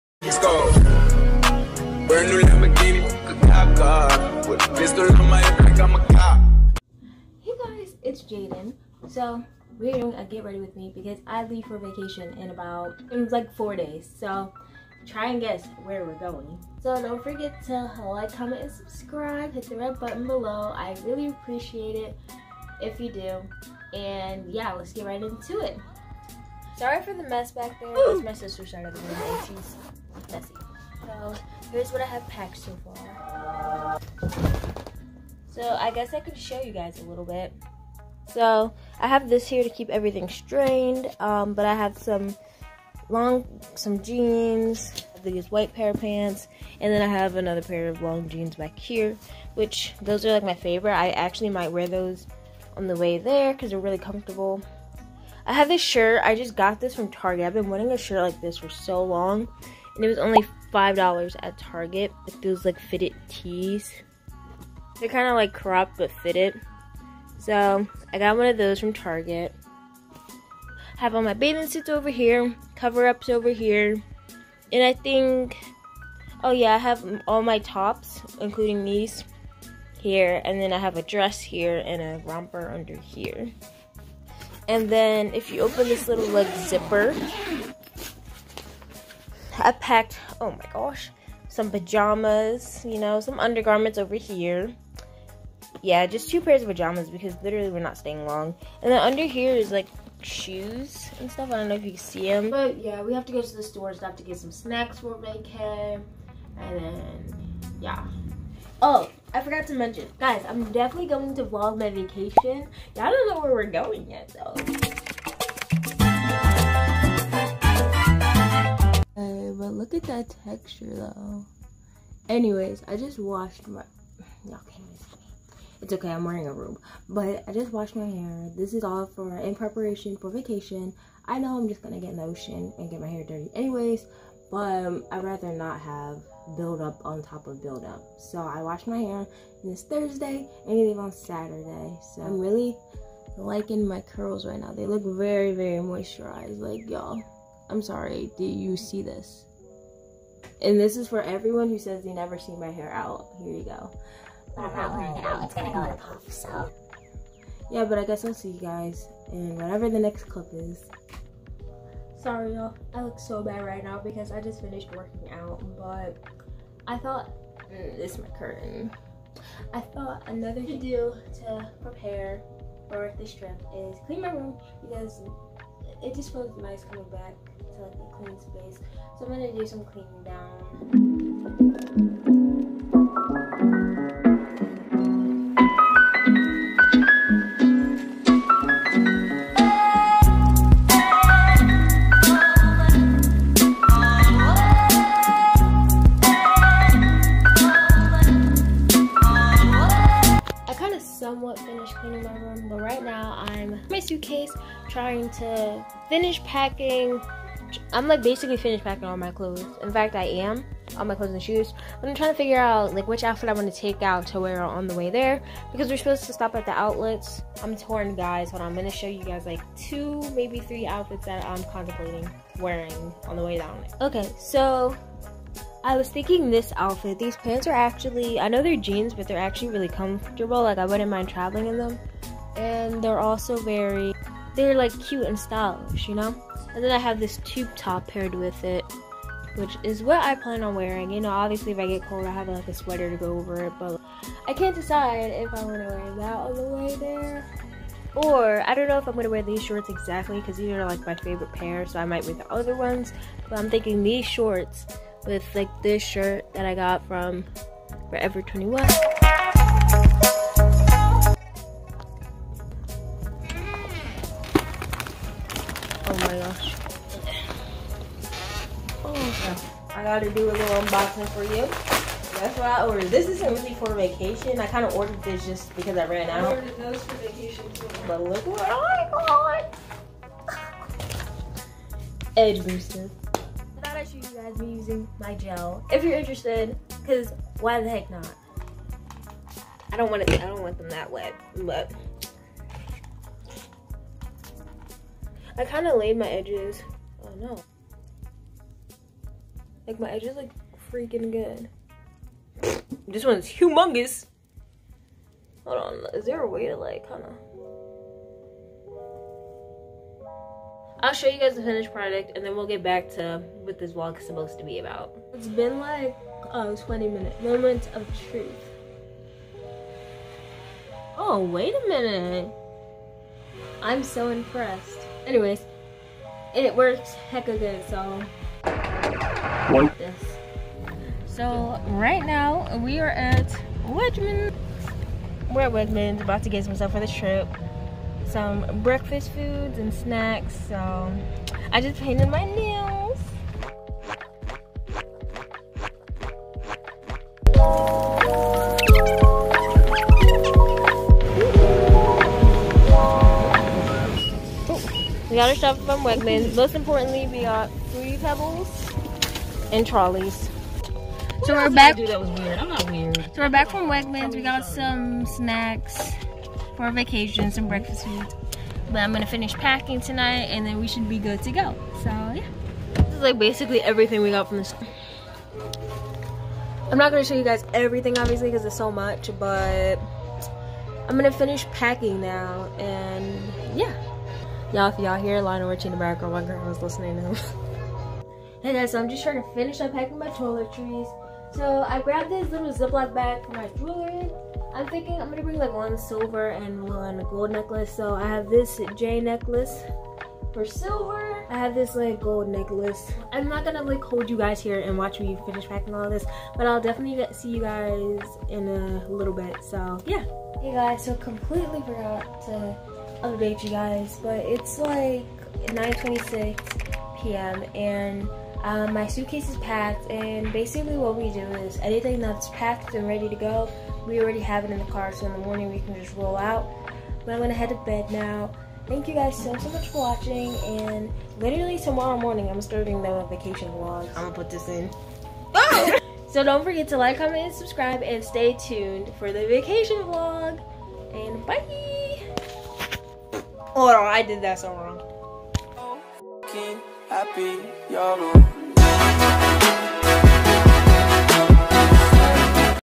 GRWM For VACATION🌴
Join me in preparing for my first vacation since 2020!!